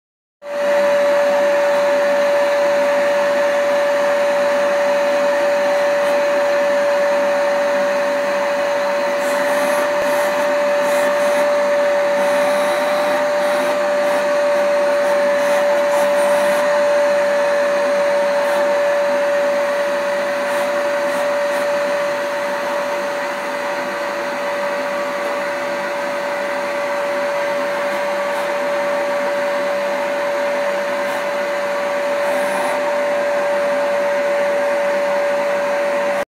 Hair Hoovering
Saving money; getting ones hair cut at home!